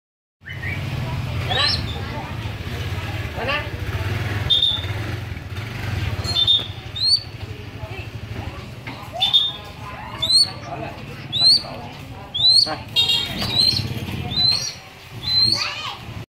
Bird Training
Filmed off my Samsung phone at various placesin Phnom Penh Cambodia